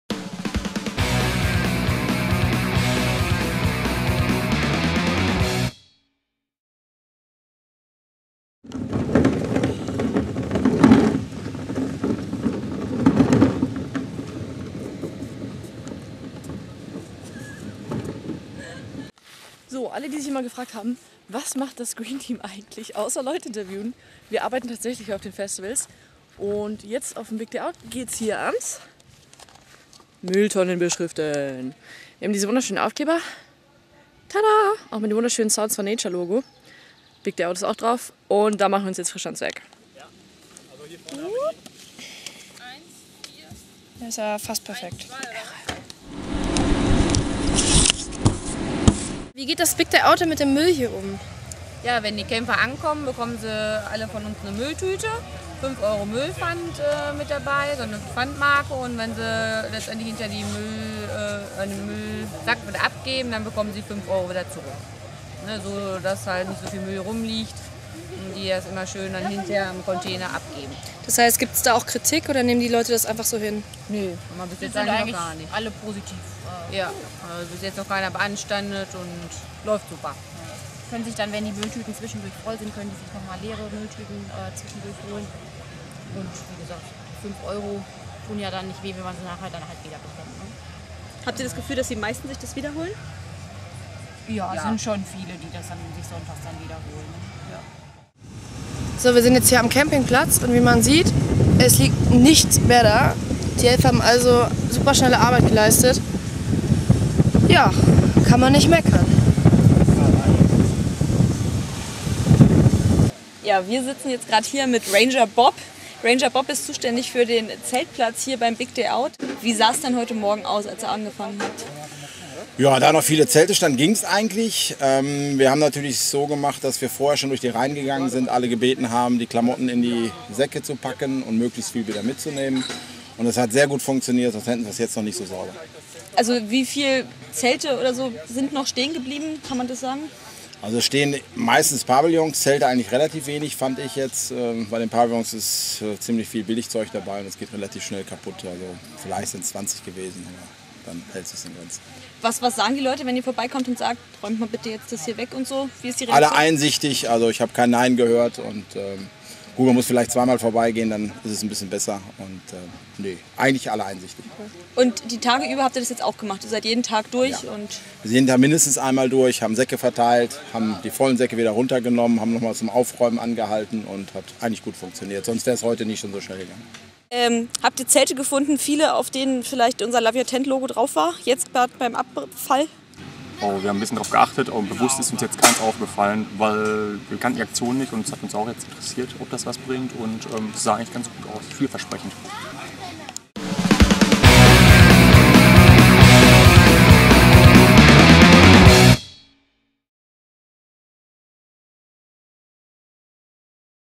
Green Team: Big Day Out - Müll und Zelte
Das Green Team filmt sich beim arbeiten. Es geht ans Mülltonnen aufstellen (woohoo)! Auch mit dabei: Ranger Bob und Ranger Bobby. Außerdem gibts gute Neuigkeiten zu Love Your Tent.